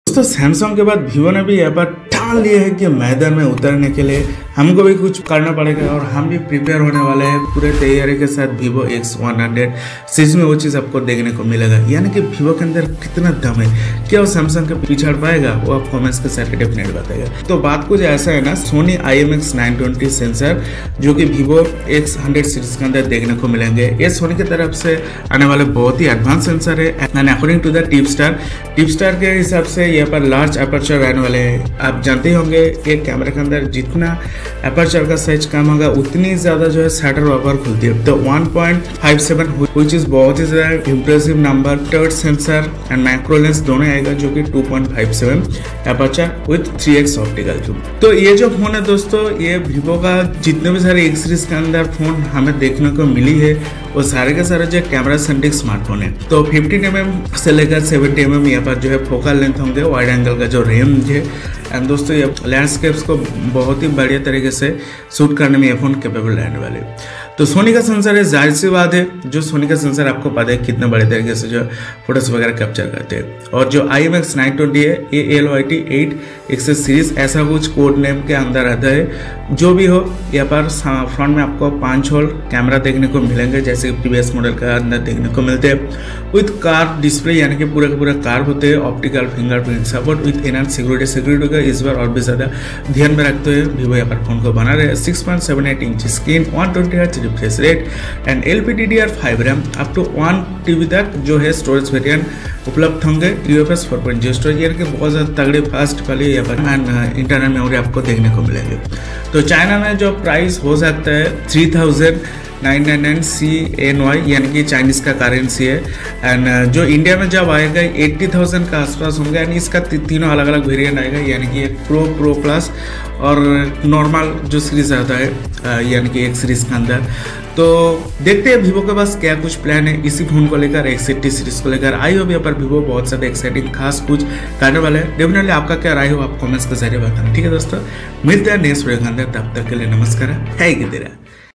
Vivo X 100 Pro + | Camera Details || Sony IMX 920 🔥 SD Gen 3 | Price ⚡️
People's Quarry :-

According to the tipster, the Vivo X100 will feature a triple rear camera system led by a Sony IMX920 sensor with a large aperture of f/1.57. It will be joined by an ultrawide sensor and a periscope tele-macro lens with an f/2.57 aperture and a 3x optical zoom.
The setup will also cover a wide range of focal lengths, from 15mm to 70mm. This will allow users to take a variety of shots, from landscapes to close-ups.
Sony IMX920” is an internal reference model name and that Sony is currently emphasizing its Lytia brand name. Thus, the final marketing name of the IMX920 may be in the “LYT8xx series”.
A1.5K BOE-centered single-hole curved screen with short-focus optical fingerprint support for enhanced security.
It will likely be a 6.78-inch screen with a 120Hz refresh rate. Likewise, the device will be powered by a Dimensity 9300 processor with up to 16GB of LPDDR5T RAM and up to 1 TB of UFS 4.0 storage.
As for the pricing, a recent listing on a Chinese e-commerce platform suggests that the Vivo X100 is expected to carry a starting price of 3,999 CNY (~$546) in China. We will learn more about the device when it launches next week.